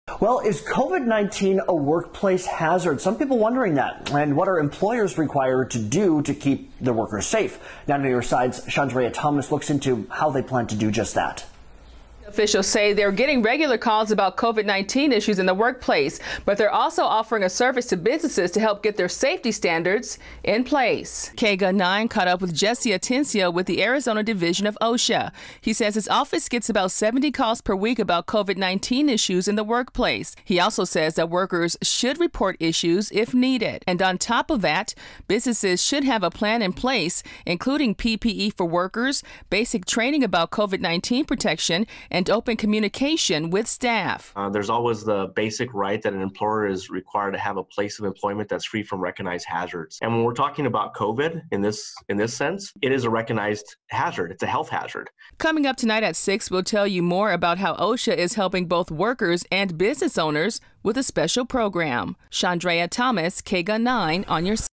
Employees returning to work during pandemic
OSHA officials say they're getting regular calls about COVID-19 issues in the workplace. They're also offering a free service to help companies improve safety standards.